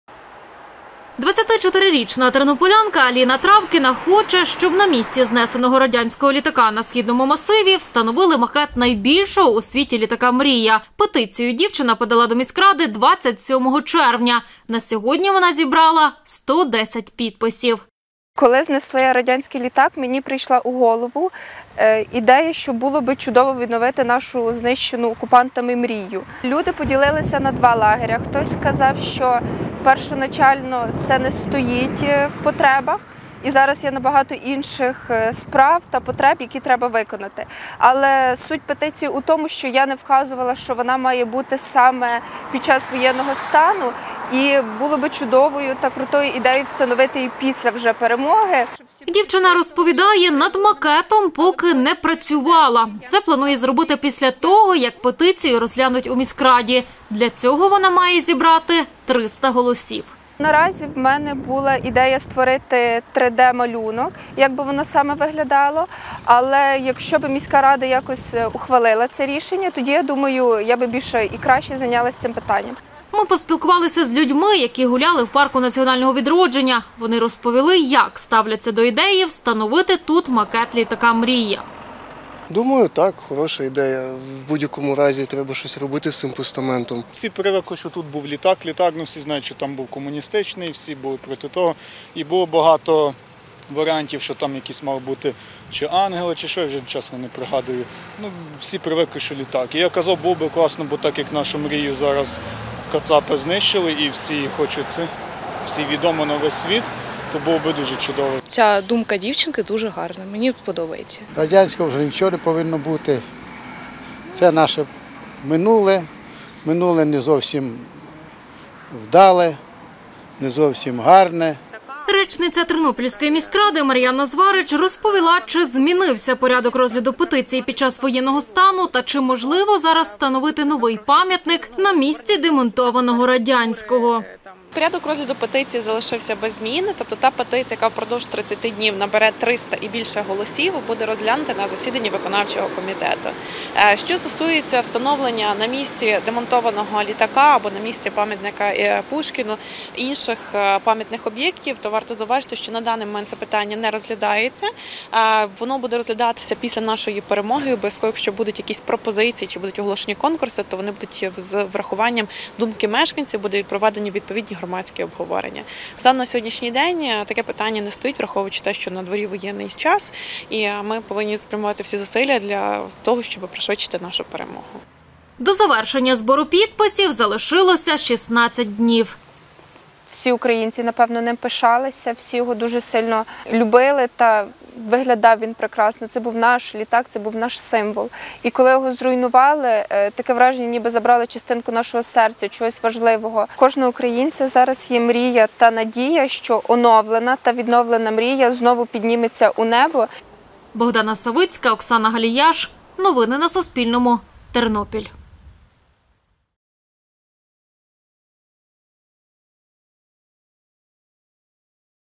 Тернополянка пропонує замість демонтованого радянського літака встановити макет "Мрії"
Тернополянка Аліна Травкіна запропонувала, щоб на місці демонтованого радянського літака на "Східному" масиві встановили макет найбільшого у світі українського літака "Мрія". Петицію 24-річна дівчина подала до міськради 27 червня.